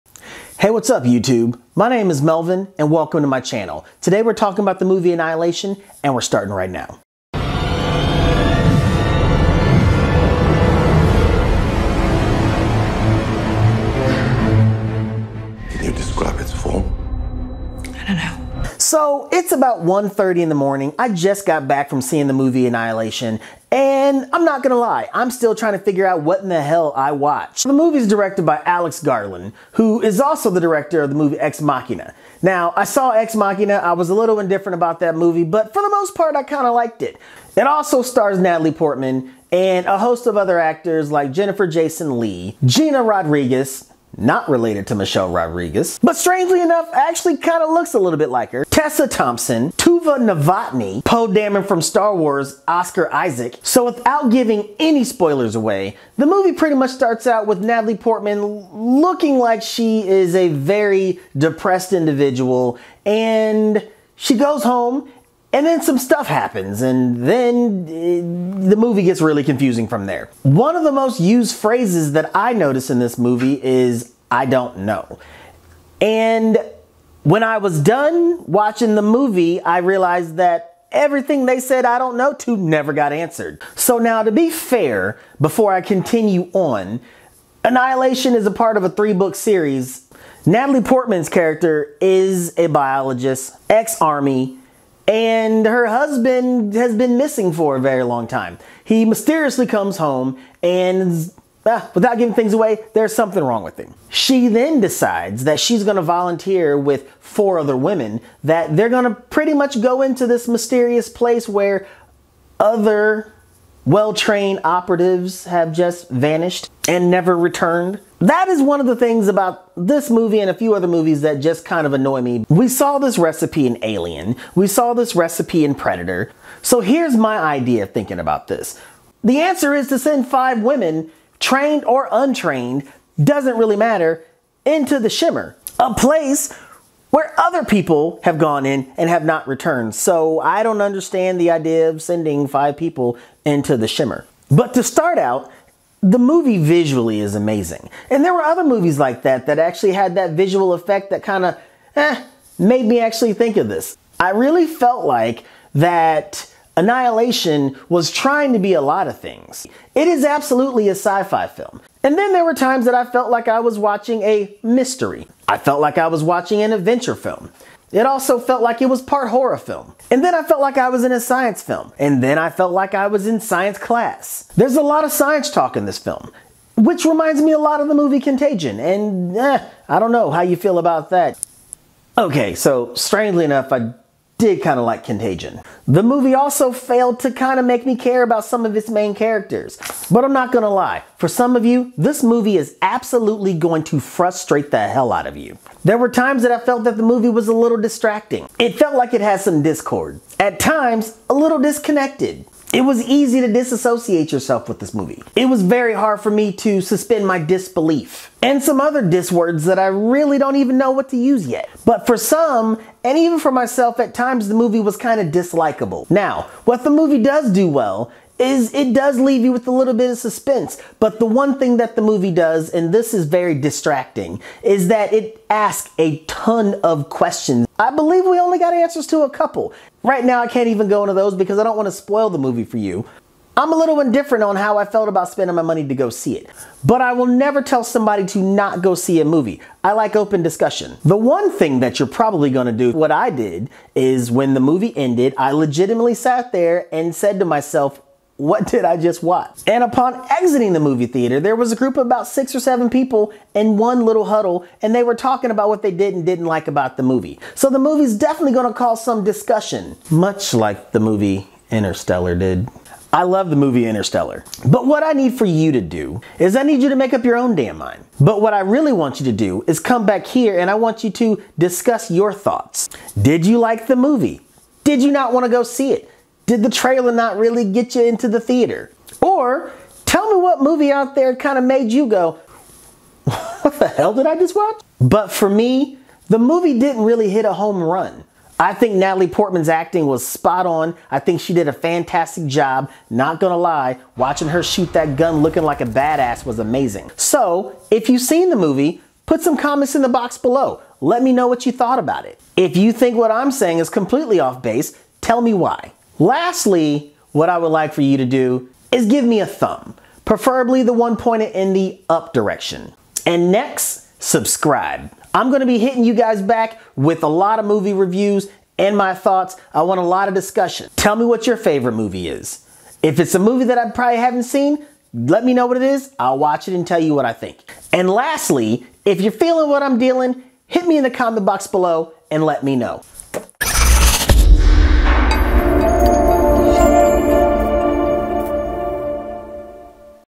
ANNIHILATION! movie review
The director of Ex Machina is back with another movie.   Check out.  what did you think? Heres my review..